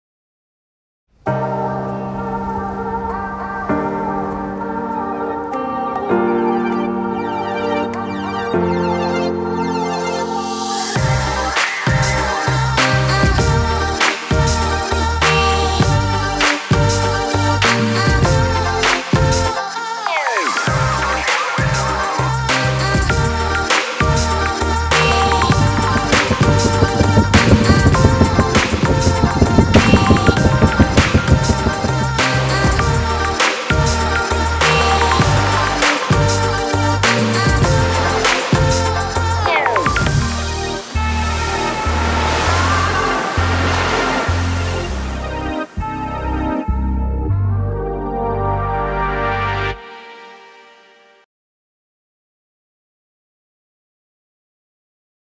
Behind the scenes with Gentleman's Journal and David Gandy London to Monaco
Follow us and David Gandy as we film 24 hours London to Monaco.